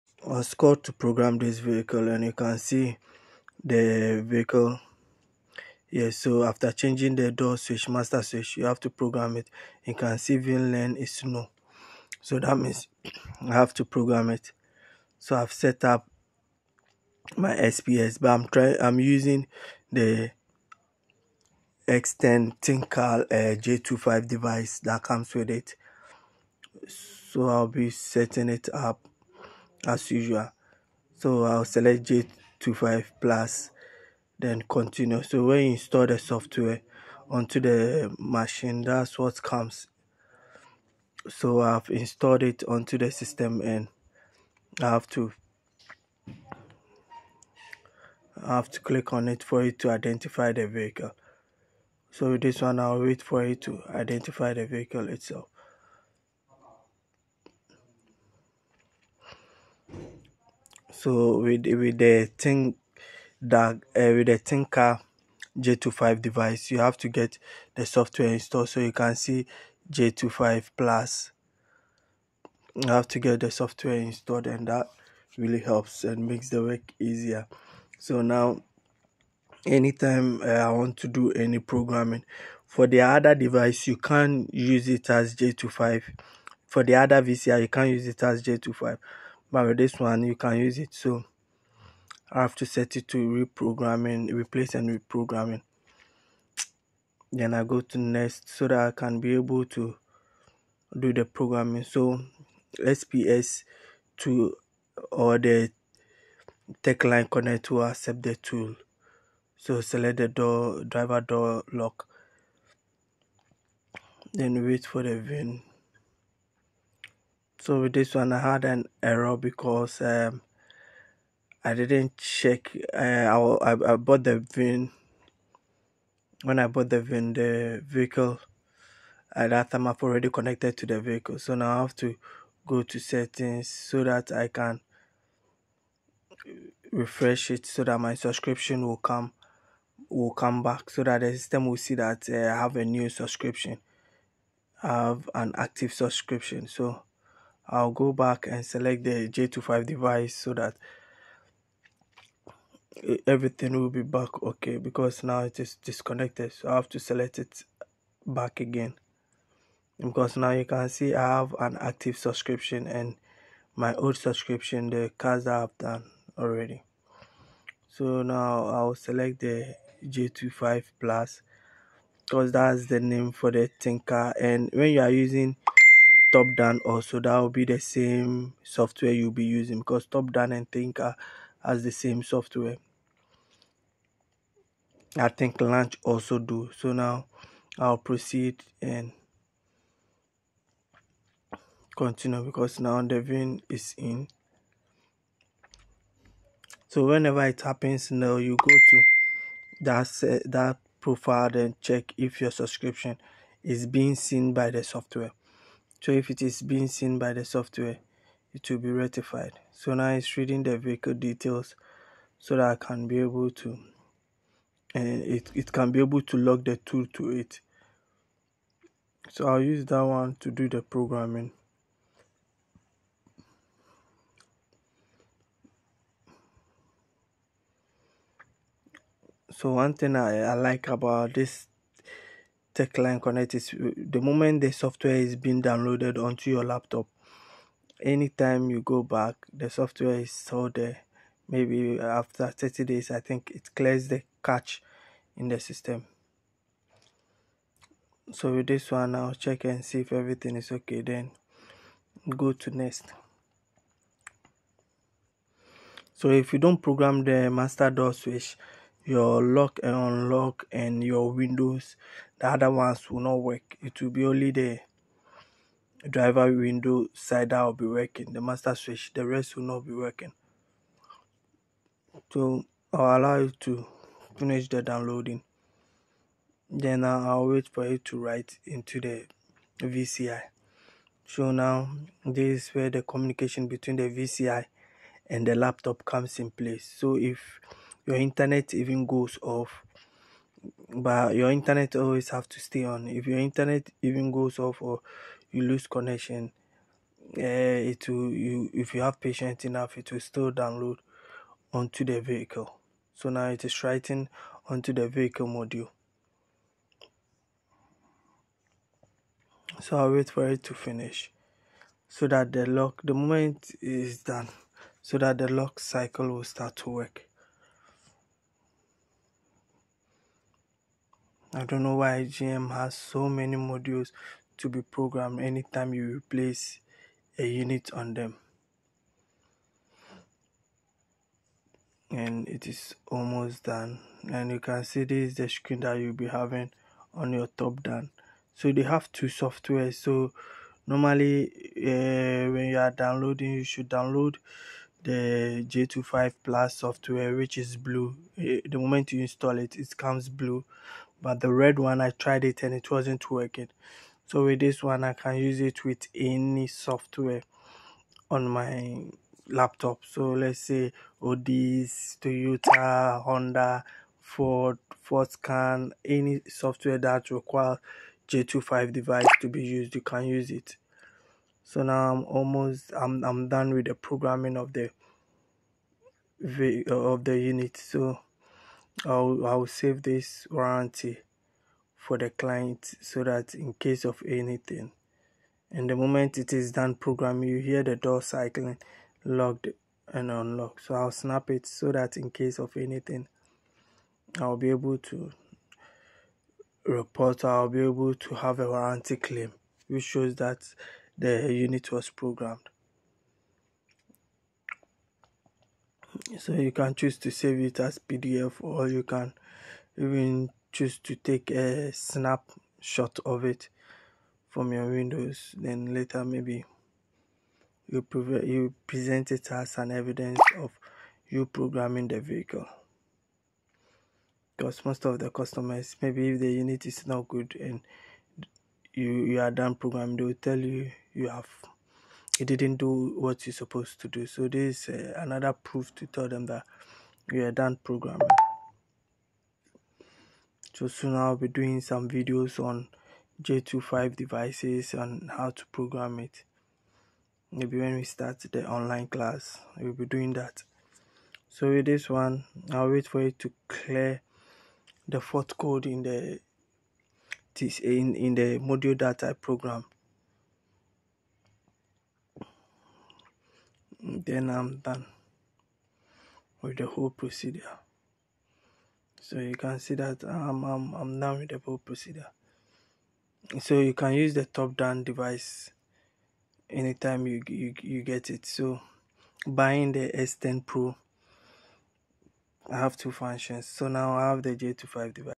Escalade: master door switch programming with thinkcar J2534 plus (TLC)
J2534 plus